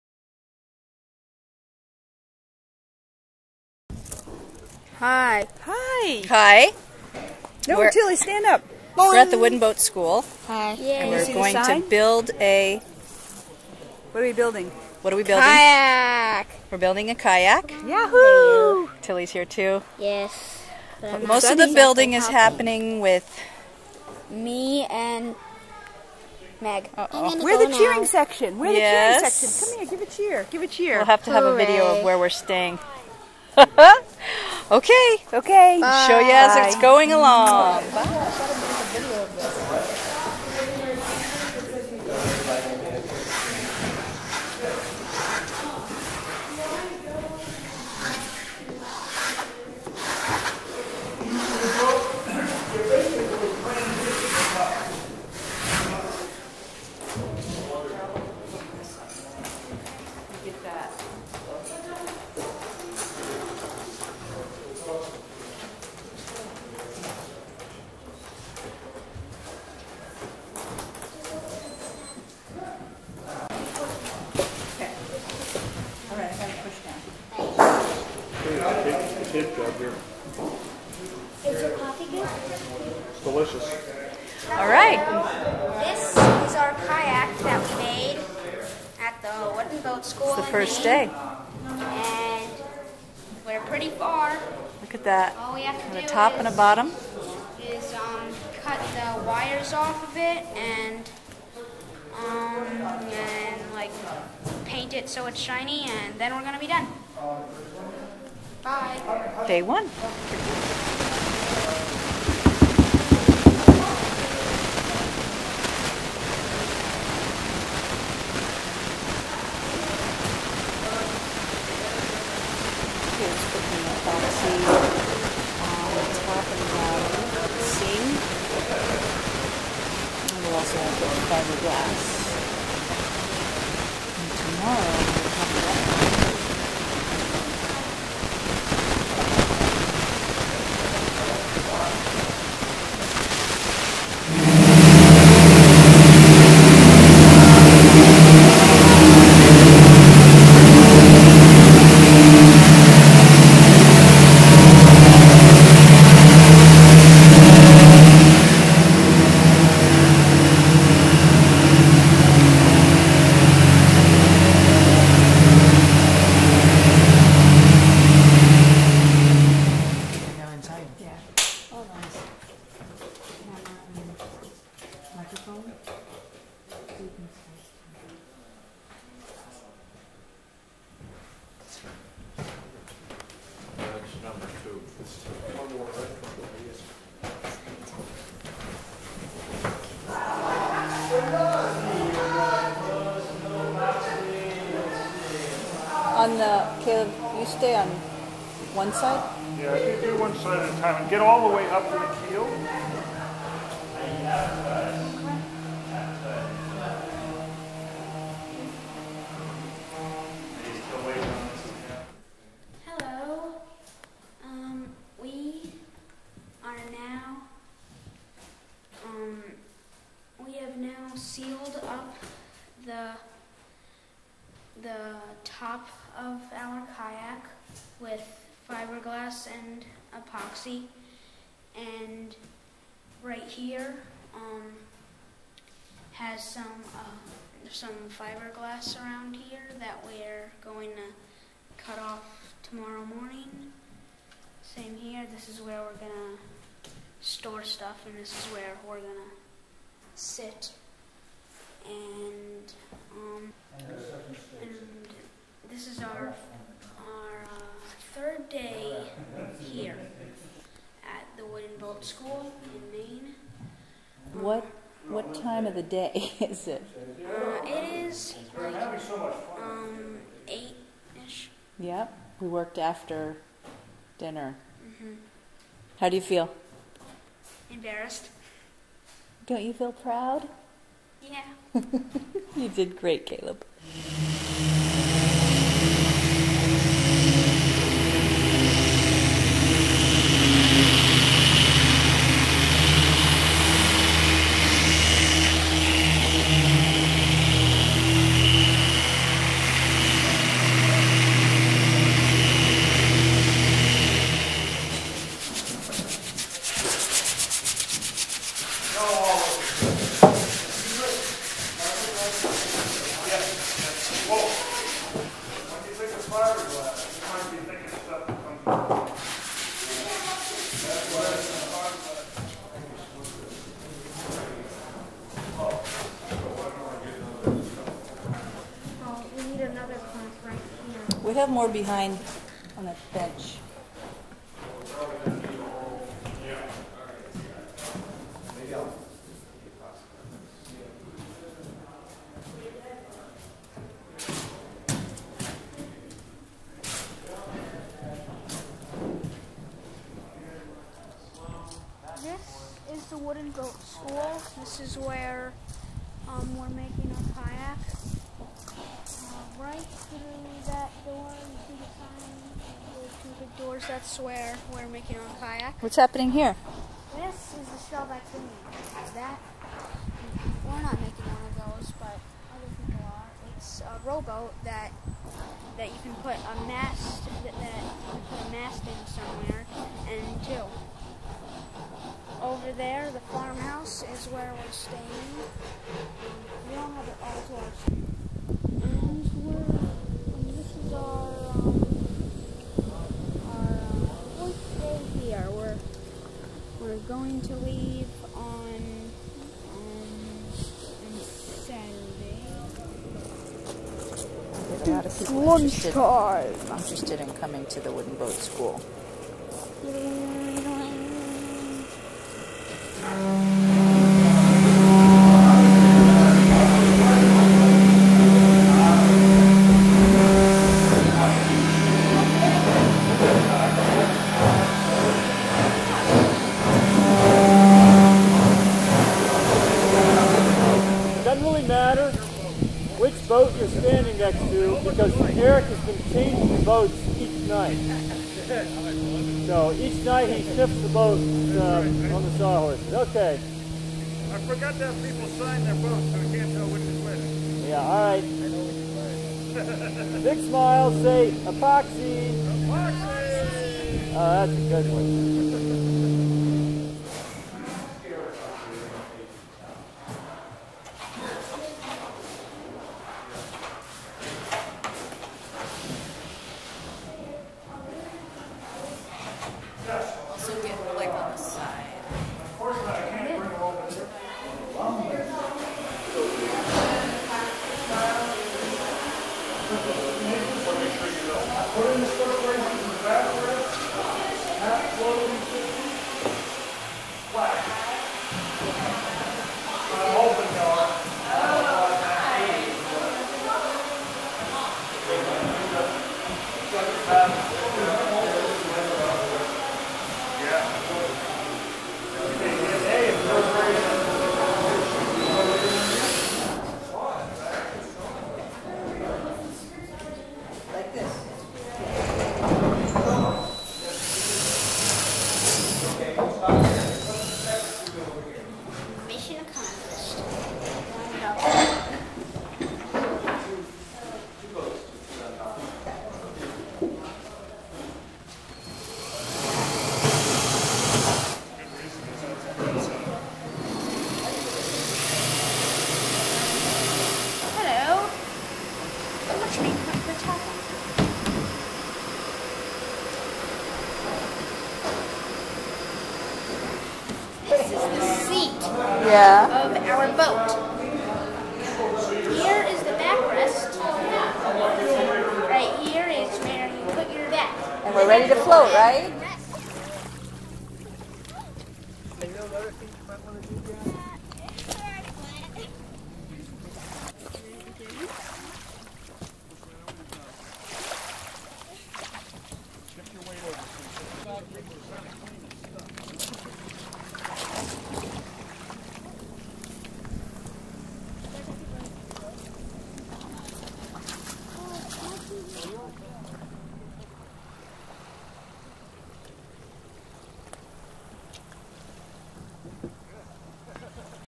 Making a Wood Duck Kayak at WoodenBoat School, Brooklin, ME
Over Family Week (Aug 4 to 9, 2008) at the WoodenBoat School, my son and I built a wood duck kayak.  We started on a Monday and had the kayak in the water by Saturday afternoon. This video documents the process of building a wooden kayak.